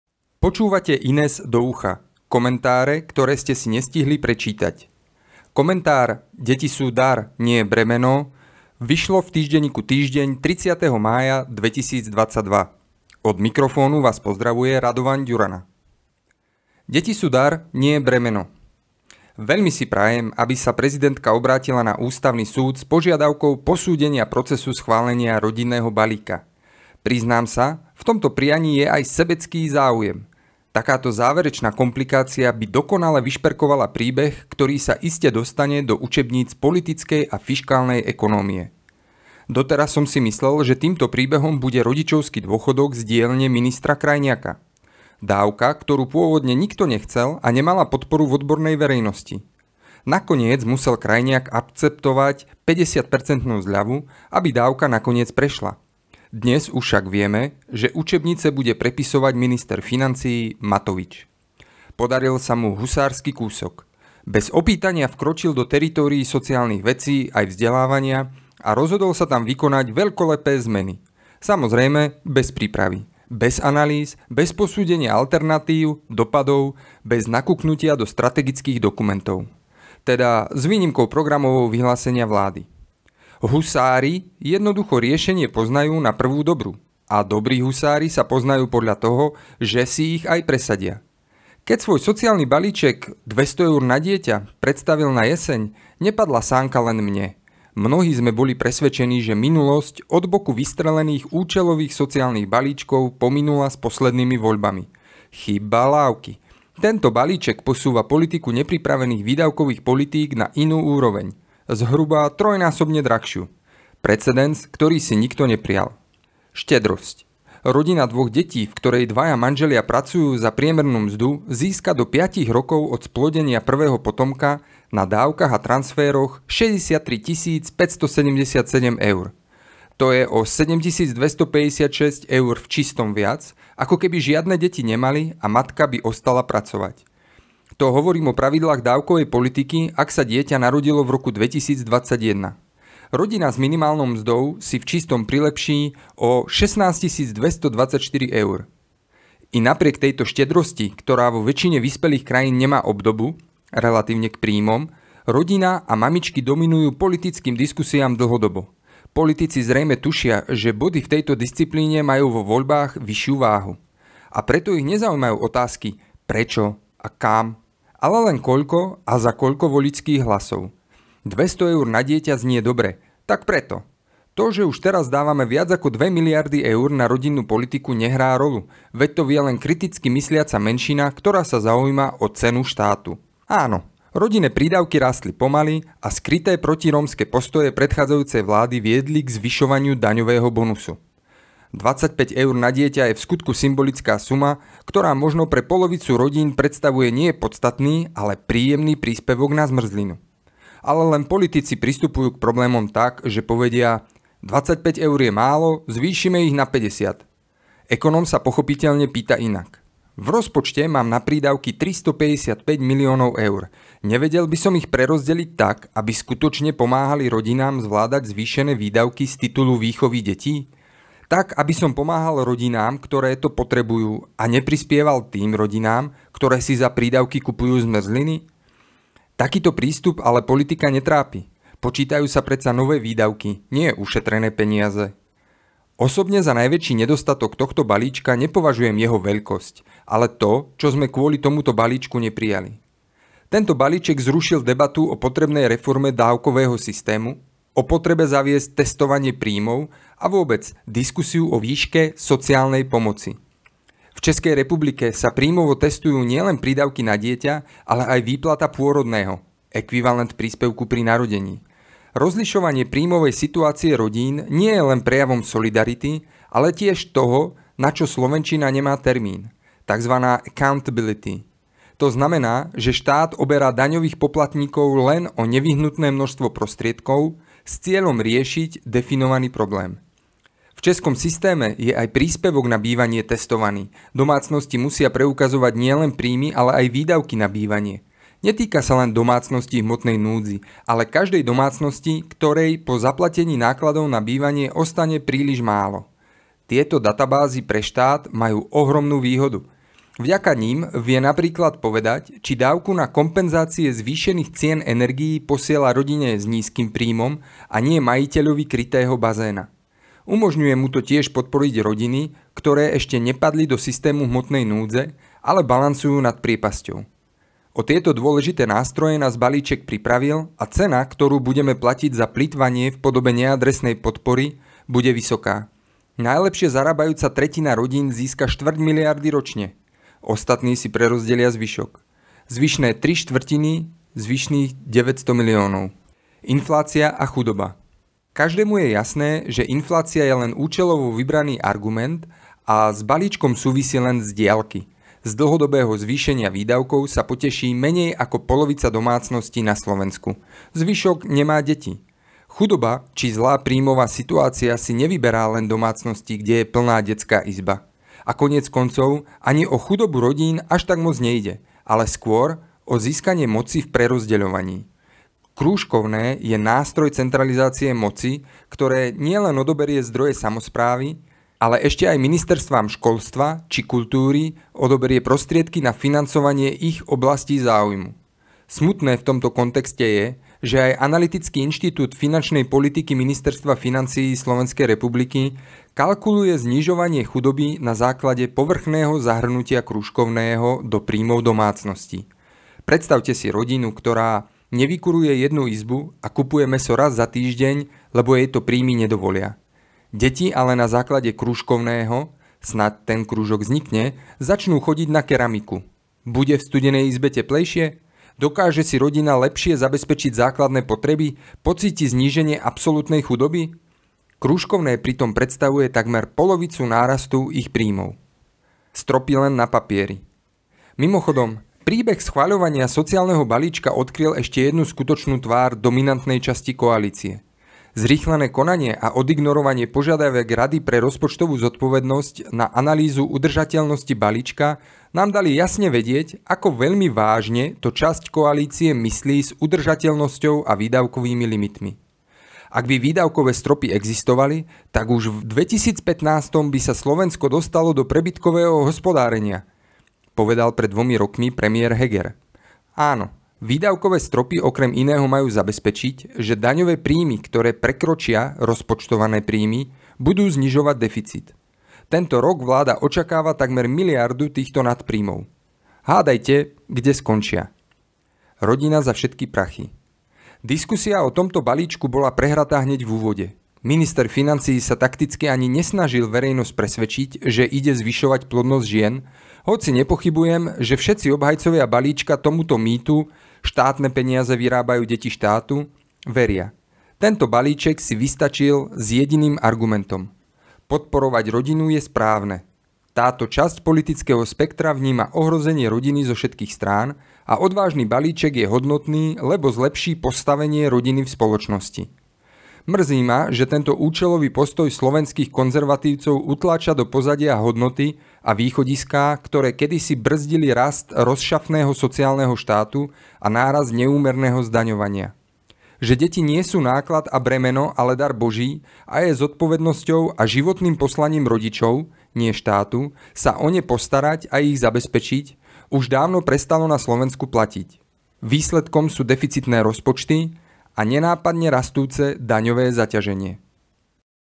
INESS do UCHA #7: Deti sú dar, nie bremeno
Mnohí sme boli presvedčení, že minulosť od boku vystrelených účelových sociálnych balíčkov pominula s poslednými voľbami. Chyba lávky, nový „prorodinný“ balíček posúva politiku nepripravených výdavkových politík na inú úroveň. Zhruba trojnásobne drahšiu. Precedens, ktorý si nikto neprial.

Politici zrejme tušia, že body v tejto disciplíne majú vo voľbách vyššiu váhu. A preto ich nezaujímajú otázky prečo? a kam? ale len koľko a za koľko voličských hlasov. Tých 200 eur na dieťa znie dobre, tak preto. To, že už teraz dávame viac ako 2 miliardy eur na rodinnú politiku, nehrá rolu, veď to vie len kriticky mysliaca menšina, ktorá sa zaujíma o cenu štátu.

Už dnes rodina 2 detí, v ktorej manželia zarábajú priemernú mzdu, získa do 5 rokov od splodenia prvého potomka o 7 256 € v čistom viac, ako keby žiadne deti nemali a matka by ostala pracovať. (*pravidlá dávkovej politiky, ak sa dieťa narodilo v roku 2021)

Od mikrofónu vás pozdravuje Radovan Ďurana.